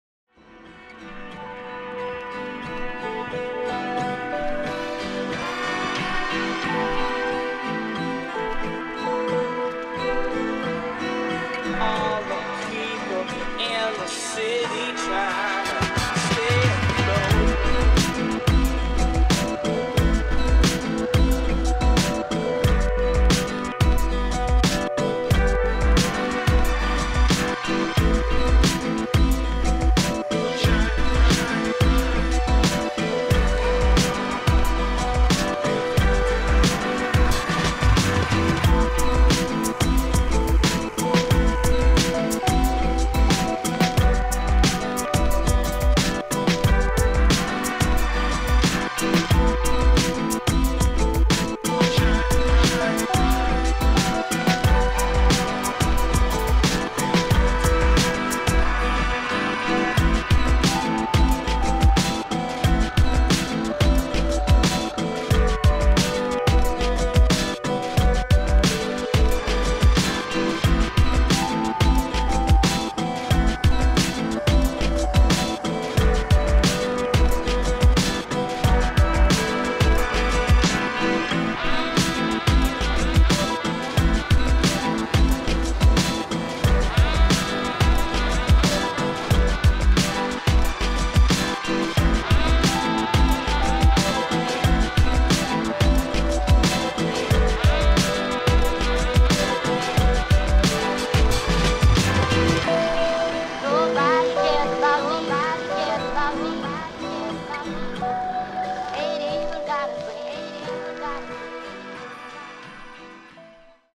Beaver Creek Snow Cat Parade
f you like fresh corduroy and snow cats, you may like watching this short video shot at Beaver Creek. Shot and edited by Nate Purcell